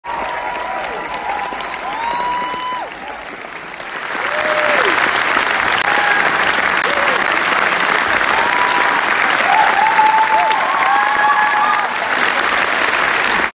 The Father and the Assassin curtain call at the National Theatre, London, May 2022
Cast members of the Father and the Assassin bow to audience at the Olivier Theatre, National Theatre, London.

for your ultimate guide to London theatre on a budget, and reviews of your favourite shows including The Father and the Assassin.